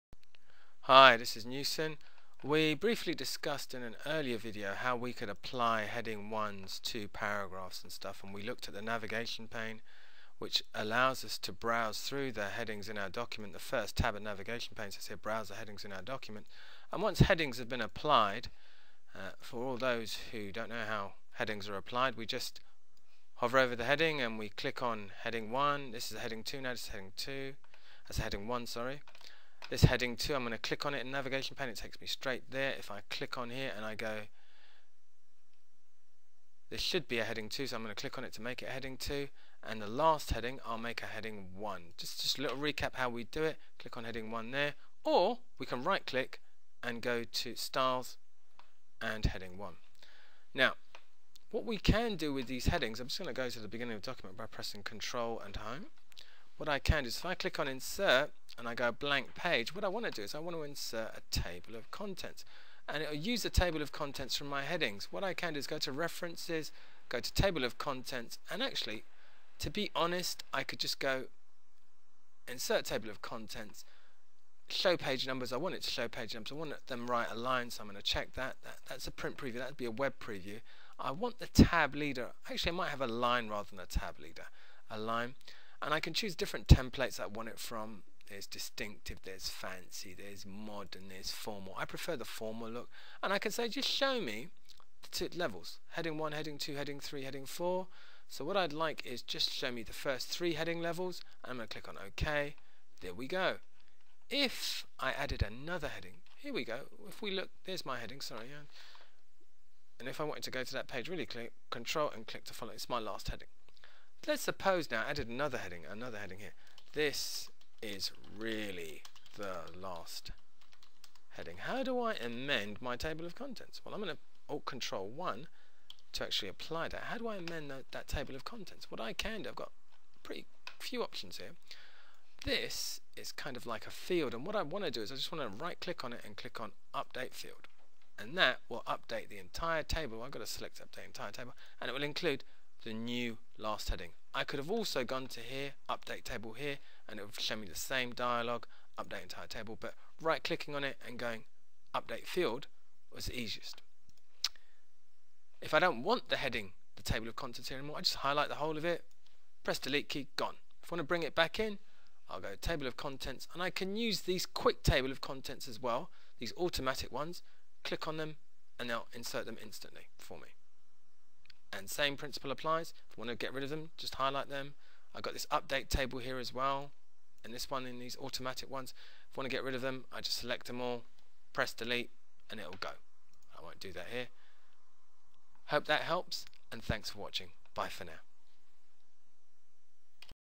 Microsoft Word: Creating Table of Contents
New Horizons Computer Learning Centre London

In this video Newson teaches us How To Create a Table of Contents using Microsoft Word document. Ideal for any schoolwork, project or novel you decide to begin!

If you would like to request a 'How To' or 'Tricks & Tips' video just send what you would like to learn to